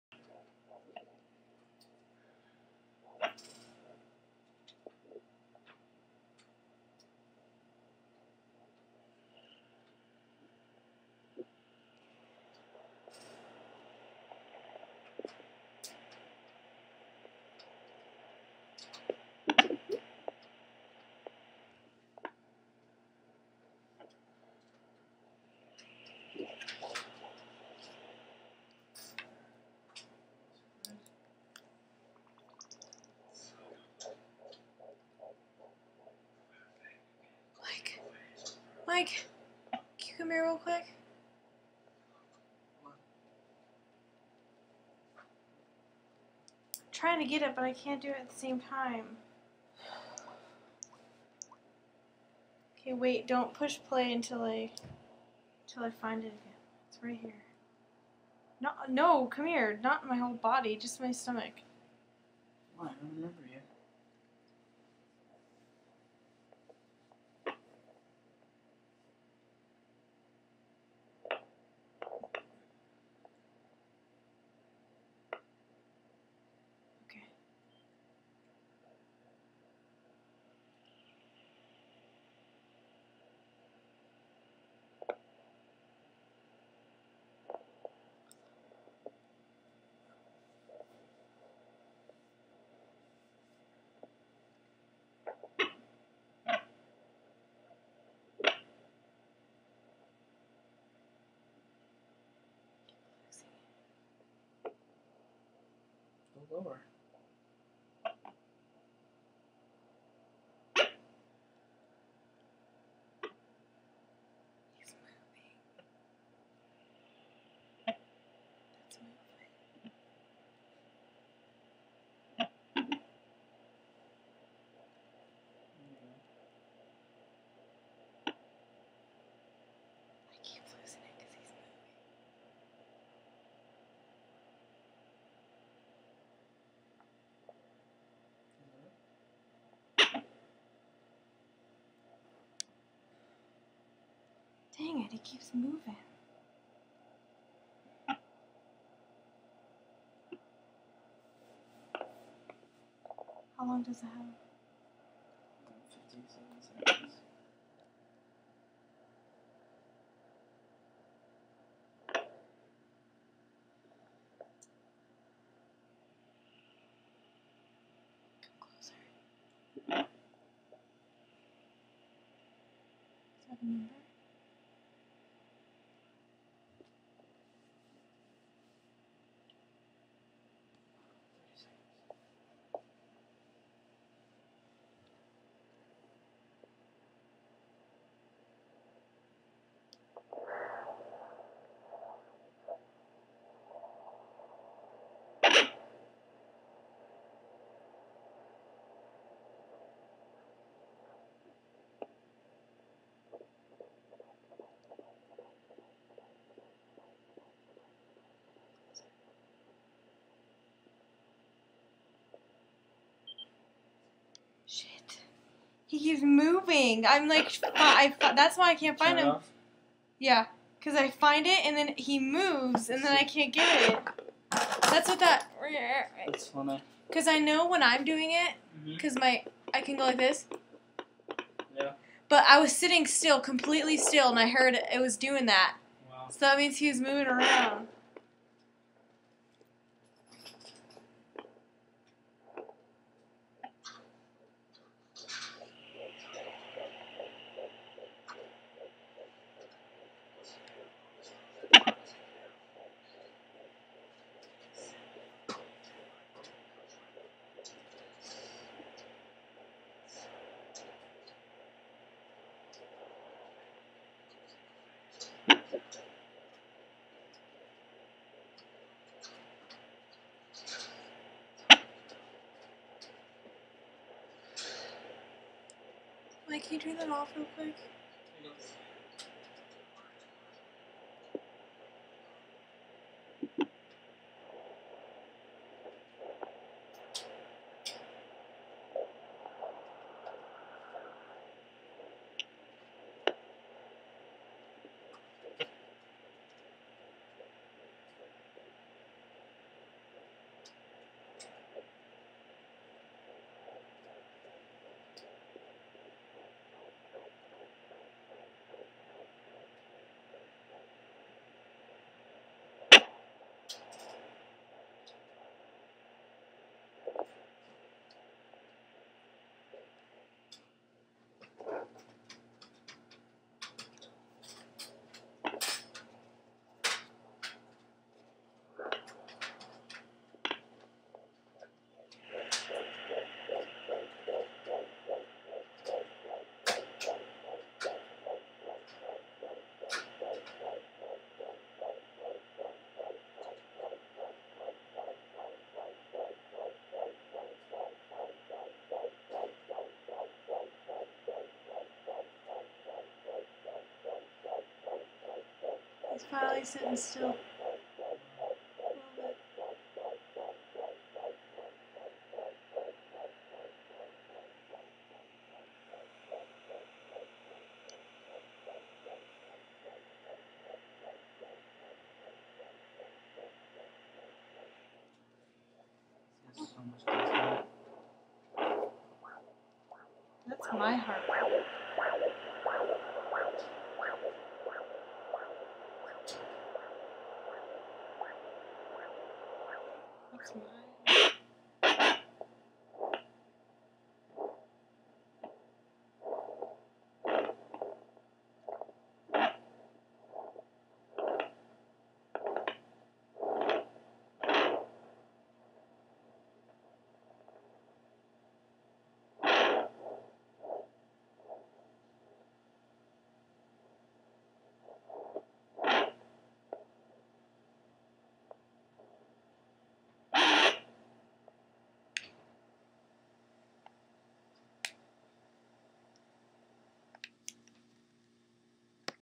Fetal Heartbeat at 15 weeks
At 5:31 you can really hear it. In the beginning the baby was moving around a lot so I kept barely getting the heartbeat, then he would move and I would lose it! lol

And YES the sound and video are a little off, so I think what you're hearing is a little before what you're seeing...

It's a Sonoline B, ordered from amazon.com. I'm 15 weeks tomorrow in this video. :) Enjoy!